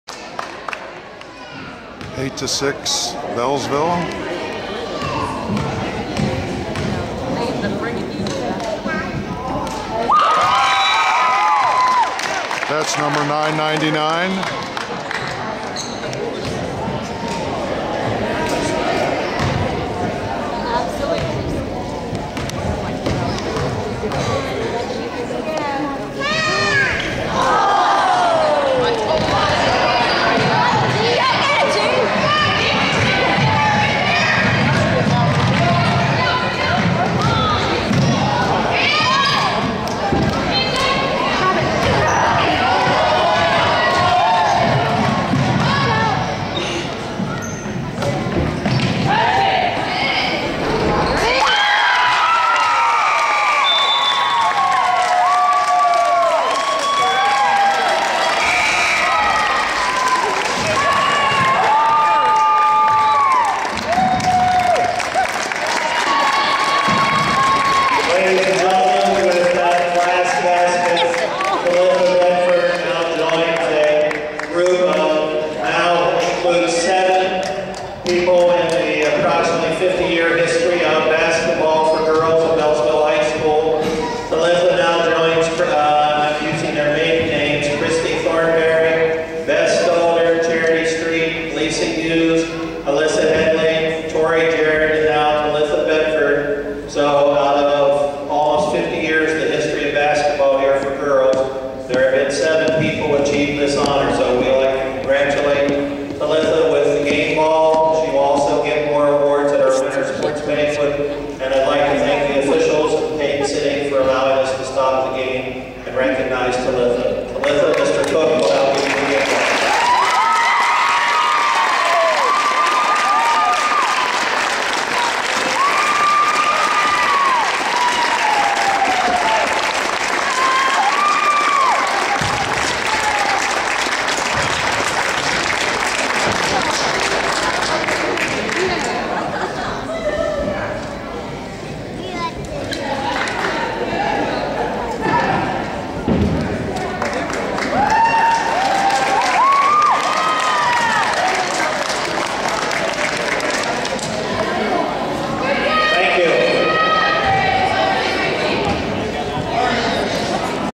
Talitha Bedford scores her 1000th point on 1/9/17 for Beallsville High Scool
Talitha Bedford #21 in White scores her 1000th  point on 1/9/17 for Beallsville High School